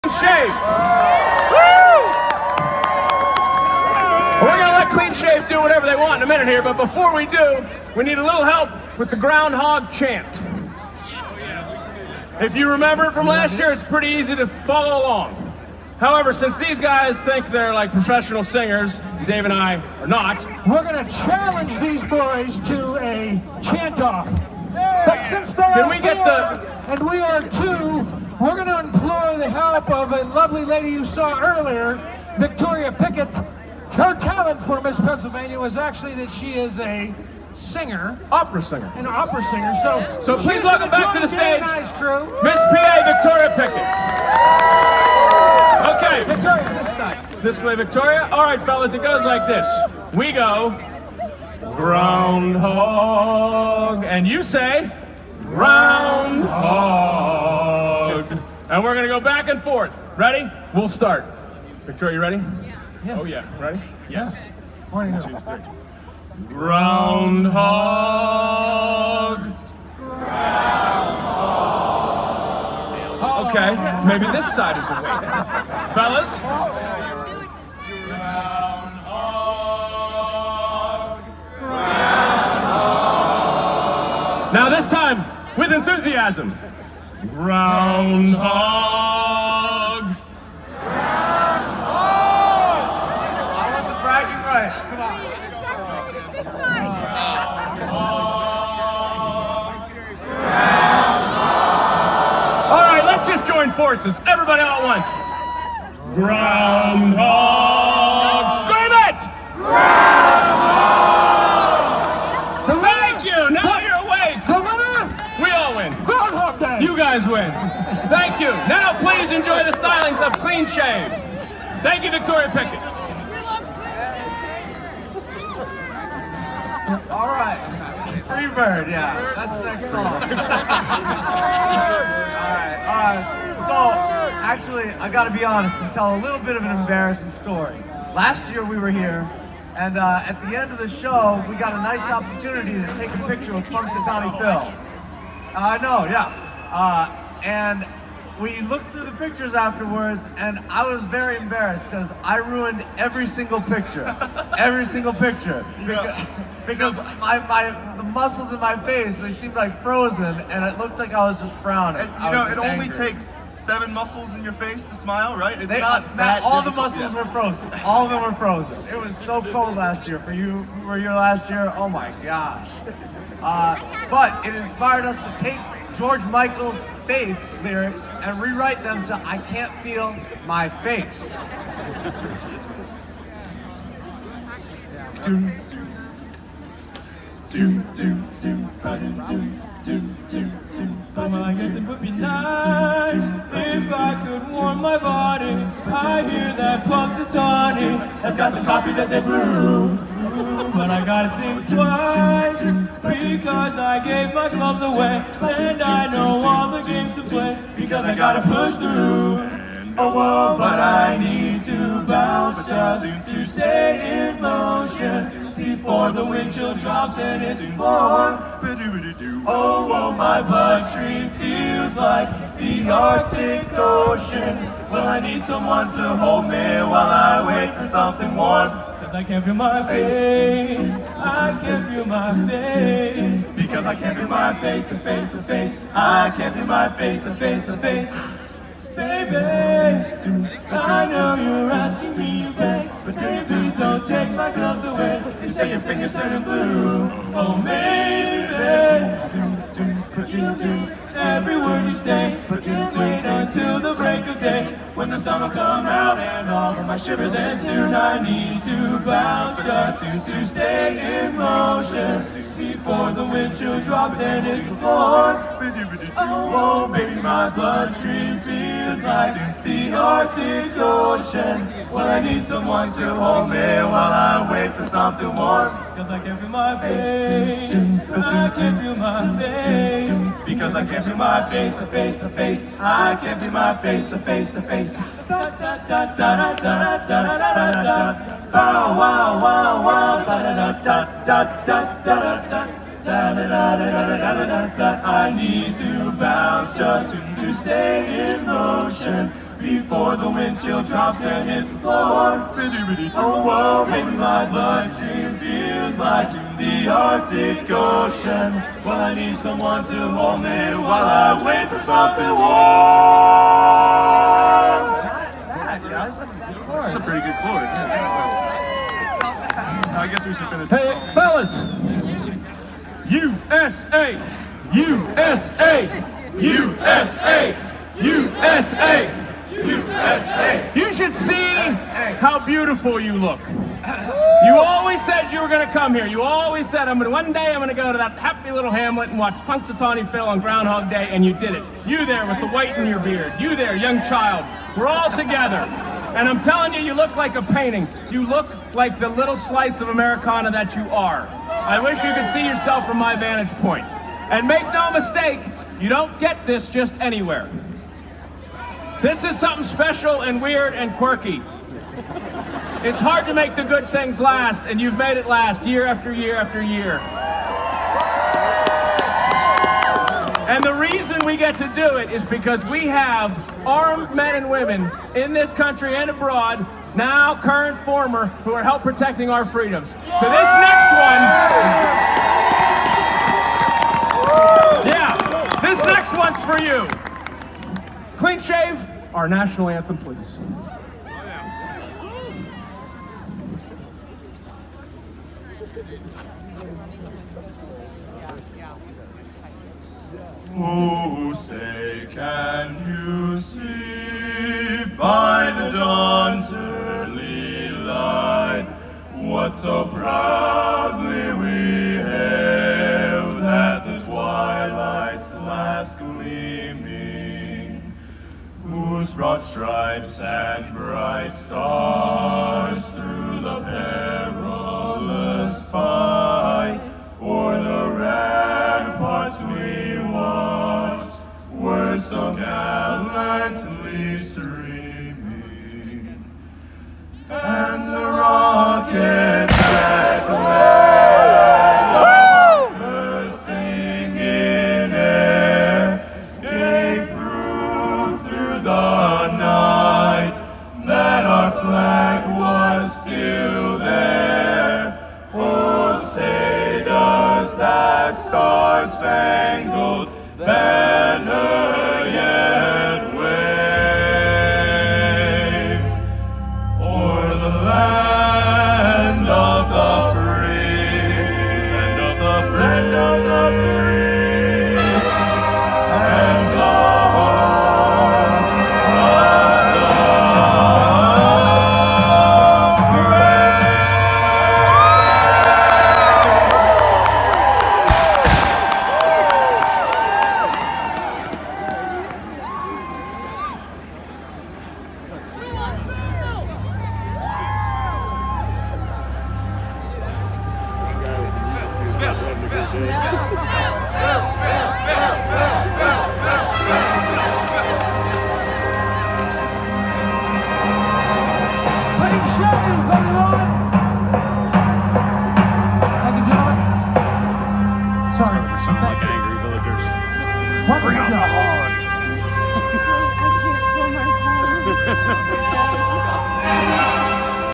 Naval Academy Barbershop Quartet II 2020 Groundhog Day
Clean Shave Set 2 (Bobby Bendik- baritone, Matt Frazier- bass, Maury- tenor, Tim- lead)